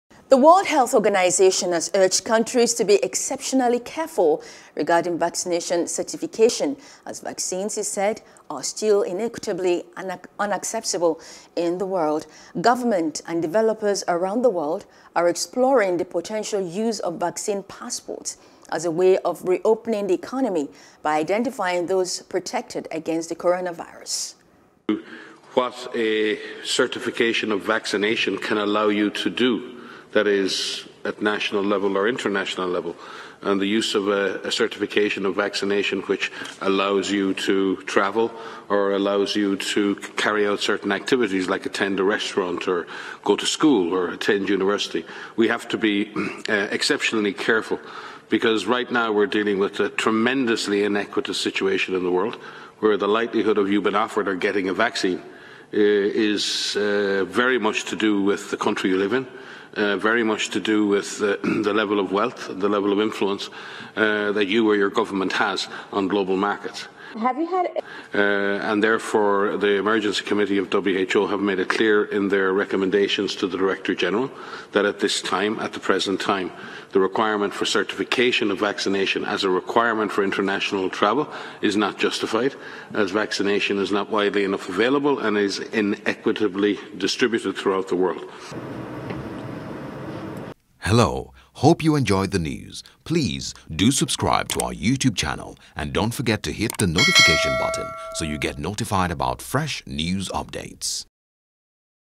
W.H.O Calls On Countries To Be Cautious Regarding Vaccines Passport | NEWS NOW
The World Health Organization has urged countries to be "exceptionally careful" regarding vaccination certification as vaccines, it said, are still inequitably accessible in the world.